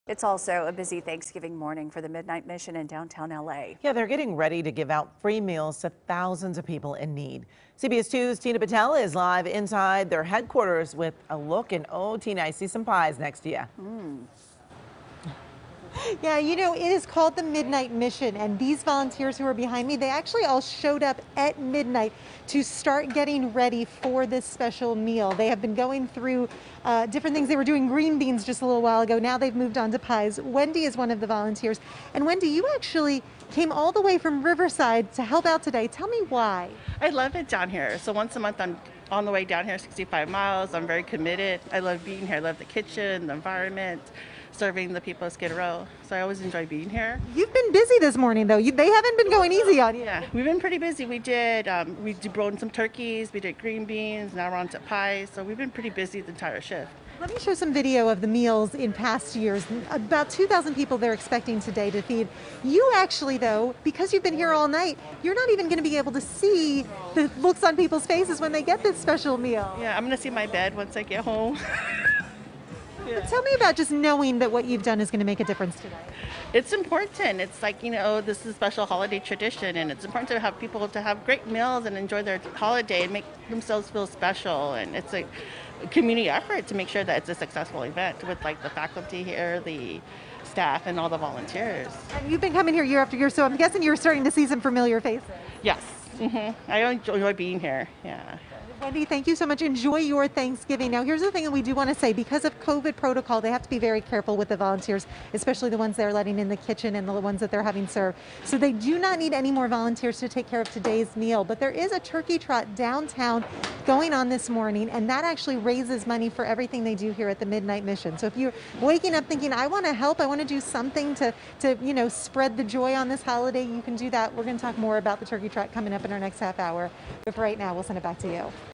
Midnight Mission, Laugh Factory Among Those Serving Thanksgiving Meals To Thousands Of Angelenos In
After taking a different form last year because of coronavirus restrictions, many Thanksgiving events to feed the needy and raise money for worthwhile causes returned Thursday to Los Angeles.